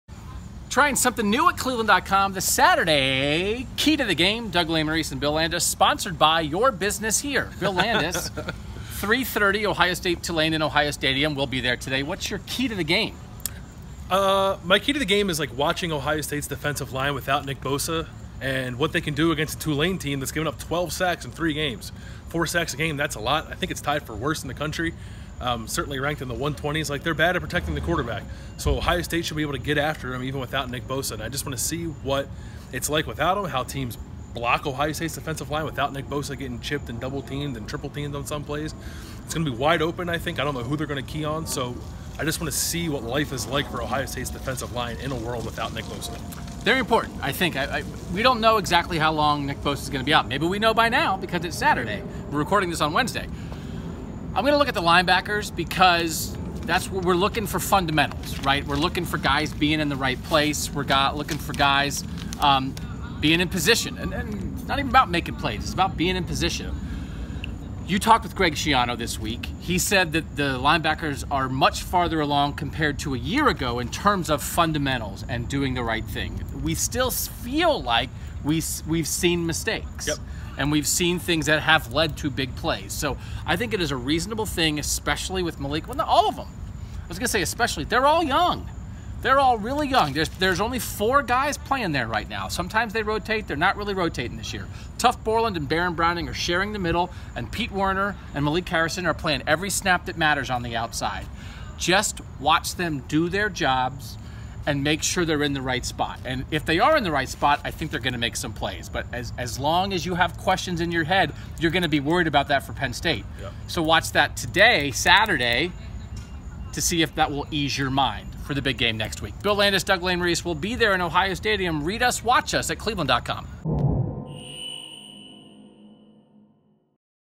Ohio State vs. Tulane: Keys to the game
Why Bill and Doug will be watching the OSU defensive line and linebackers.